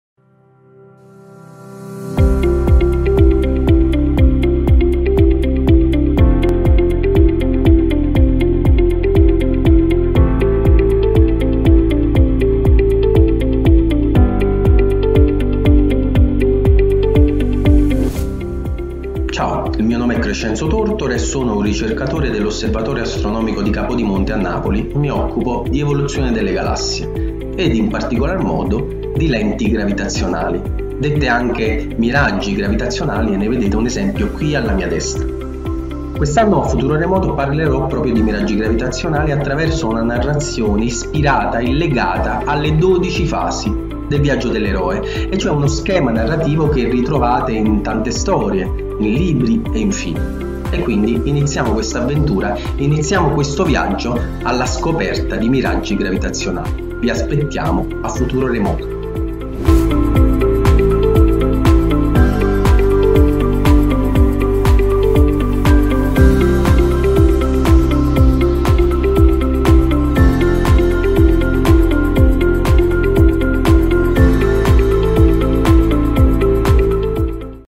CRESCENZO TORTORA per Futuro Remoto 2021 · / Tran · si · zió · ni /:
𝗔𝗹𝗹𝗮 𝗿𝗶𝗰𝗲𝗿𝗰𝗮 𝗱𝗶 𝗺𝗶𝗿𝗮𝗴𝗴𝗶 𝗴𝗿𝗮𝘃𝗶𝘁𝗮𝘇𝗶𝗼𝗻𝗮𝗹𝗶: 𝘂𝗻'𝗮𝘃𝘃𝗲𝗻𝘁𝘂𝗿𝗮 𝗿𝗮𝗰𝗰𝗼𝗻𝘁𝗮𝘁𝗮 𝗮𝘁𝘁𝗿𝗮𝘃𝗲𝗿𝘀𝗼 𝗶𝗹 𝗩𝗶𝗮𝗴𝗴𝗶𝗼 𝗱𝗲𝗹𝗹'𝗘𝗿𝗼𝗲
(cod. F176)

𝗔 𝗰𝘂𝗿𝗮 𝗱𝗲𝗹𝗹’Istituto Nazionale di Astrofisica_INAF-Osservatorio Astronomico di Capodimonte

𝗖𝗼𝗻:

𝘊𝘳𝘦𝘴𝘤𝘦𝘯𝘻𝘰 𝘛𝘰𝘳𝘵𝘰𝘳𝘢, Istituto Nazionale di Astrofisica_INAF-Osservatorio Astronomico di Capodimonte

La Relatività Generale ha rivoluzionato la nostra comprensione dell’Universo, descrivendo come i campi gravitazionali distorcono i percorsi della luce, deformando lo spazio-tempo e generando dei fenomeni astrofisici spettacolari, chiamati lenti gravitazionali, dei veri e propri miraggi creati dalla gravità. Quindi, se la luce di una sorgente lontanissima passa in prossimità di una più vicina, chiamata lente, l’immagine di quella sorgente lontana viene moltiplicata, amplificata e distorta, formando addirittura archi e anelli. Questa attività si propone di descrivere questa transizione attraverso uno story-telling basato sul Viaggio dell’Eroe, quella struttura narrativa che troviamo in antichi miti, fino a libri e film. Matrix e il Signore degli anelli sono solo alcuni esempi di questa narrazione, fatta di 12 fasi.

𝟮𝟱 𝗻𝗼𝘃𝗲𝗺𝗯𝗿𝗲 𝗮𝗹𝗹𝗲 𝟬𝟵:𝟬𝟬 · In presenza